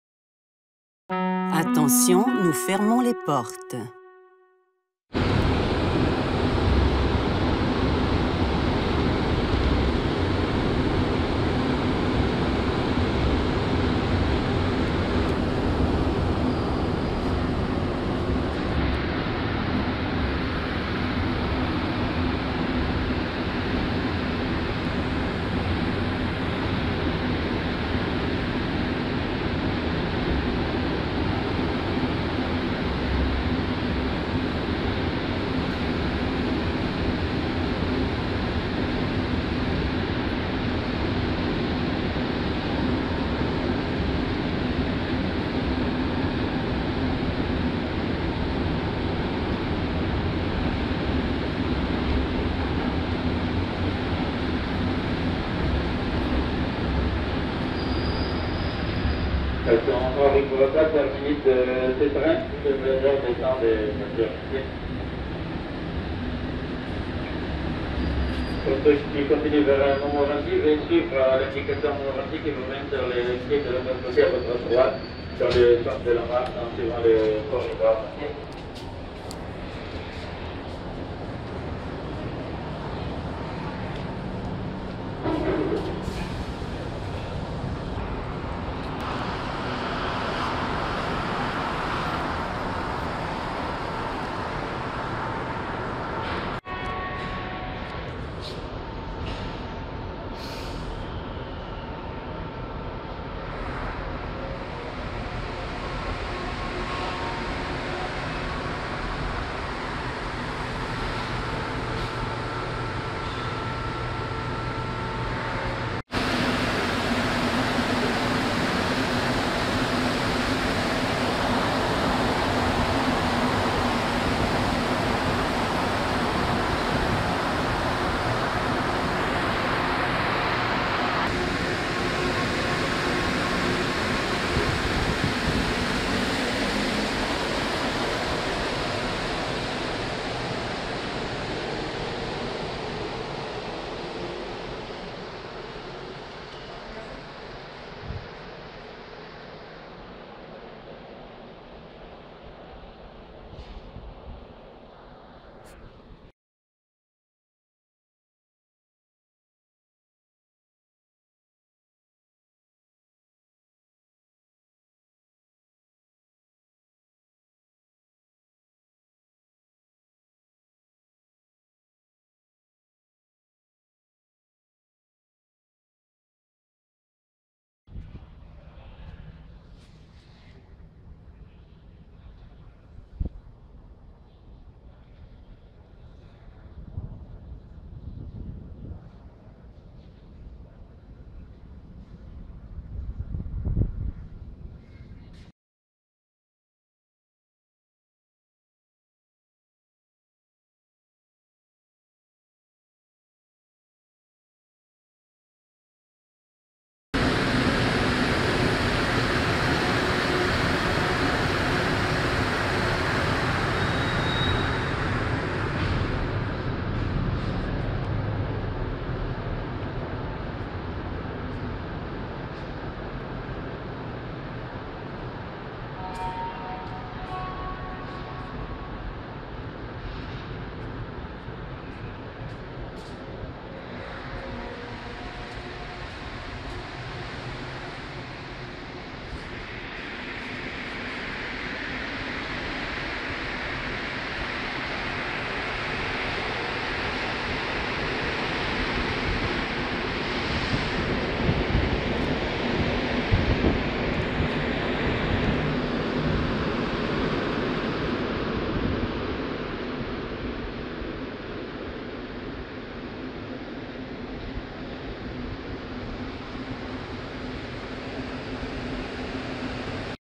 Train Going out of Service at Henri-Bourassa - Montreal Metro
The subway train at Montreal's Metro goes out of service at the Henri-Bourassa station. It unloads passengers on the platform reserved only for out-of-service trains. The driver asks passengers to walk to the platform where an in-service train will pick up the passenger.